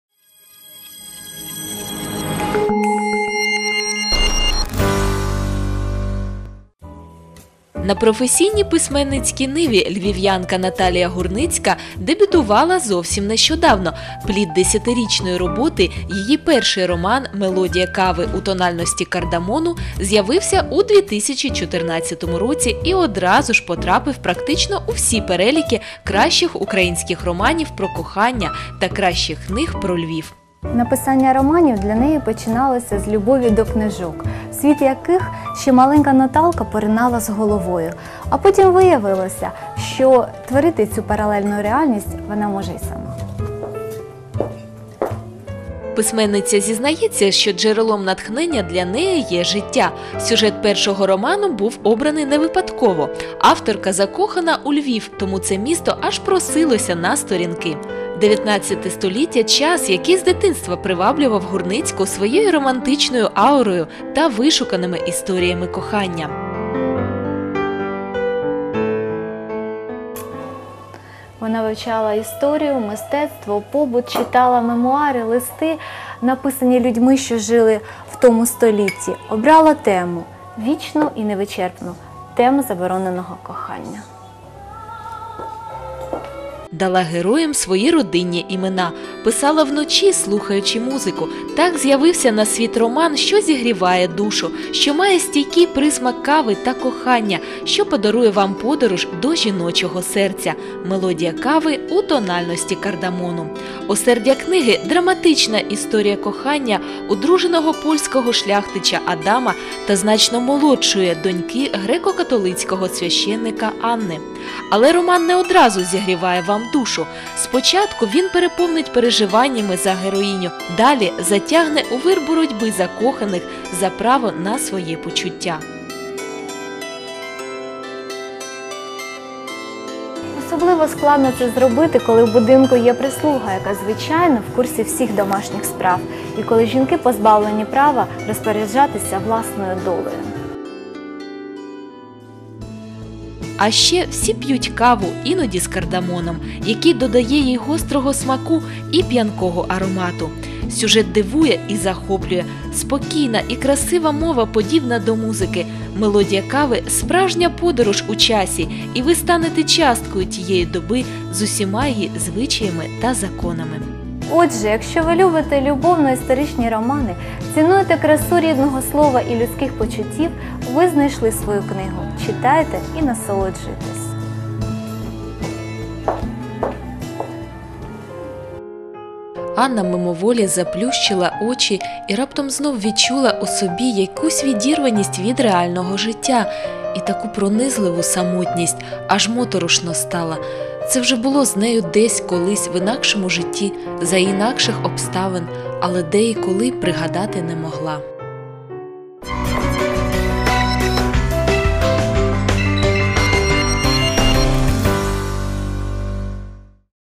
Тест 99. Наталія Гурницька
Сьогодні рубрика “Тест 99” присвячена книзі Наталії Гурницької “Мелодія кави у тональності кардамону”. Це драматична історія кохання юної дівчини і одруженого польського шляхтича. Події розгортаються в XIX у Львові.

Автор - Тетяна Гервасьєва.
Редактор — Ольга Храпенко.